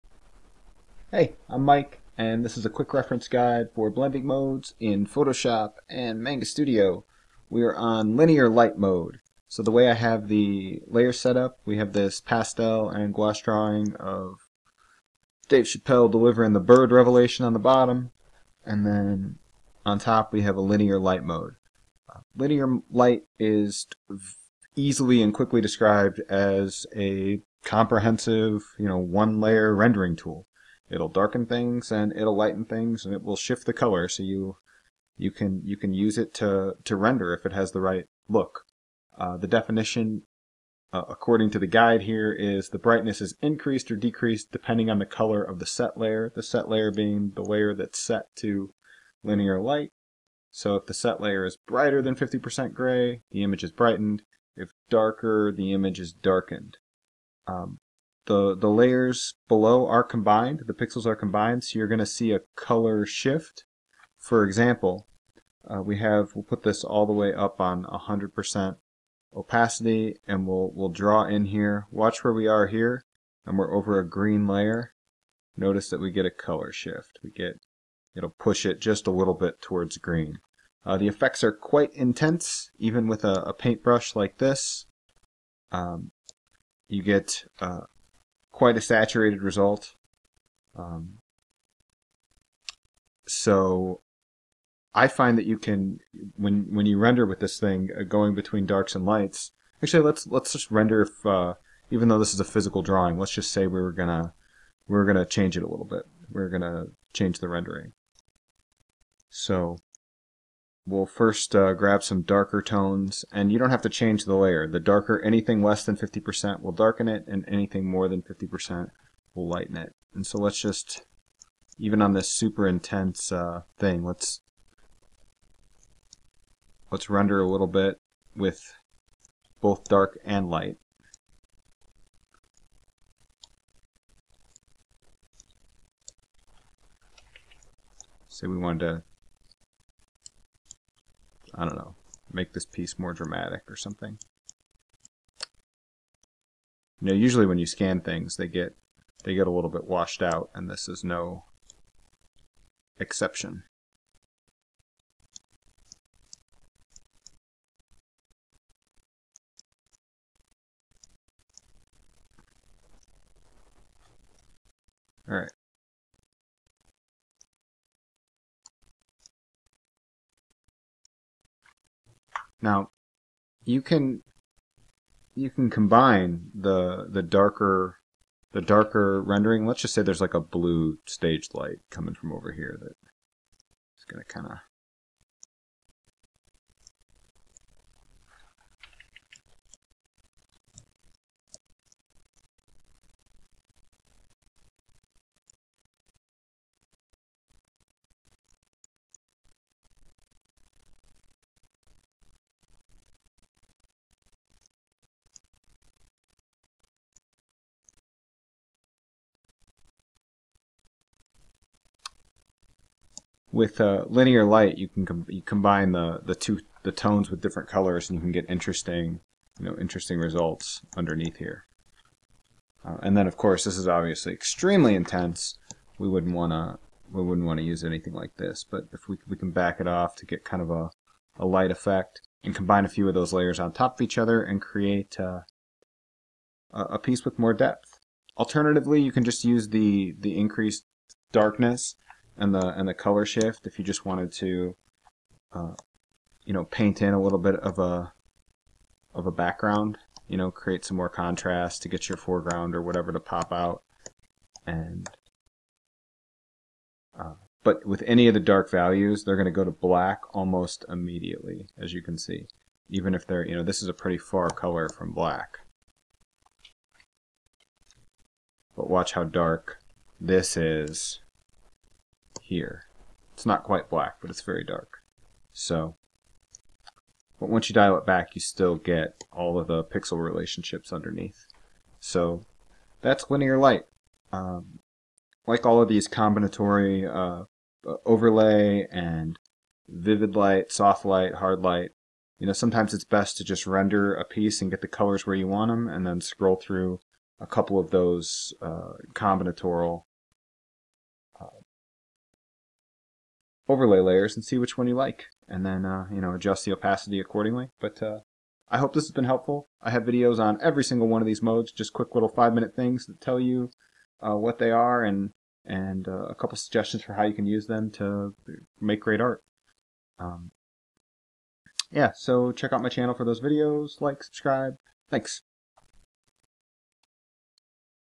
Photoshop and Clip Studio Blending Mode Reference Guide - Linear light
his video series give a quick explanation of every single blending mode available in both Photoshop and Clip Studio Paint.  There is a technical explanation of what the mode does followed by a quick demonstration using my own artwork.

Please note these videos are intended as an overview of the modes from a painting/drawing perspective and not how they can be used for photo-editing and graphic design.